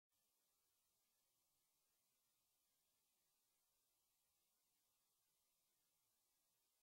Audacious Effort
Joe Salem thinks its a 5 a side and gets bored, so he just punts it. Decent effort though.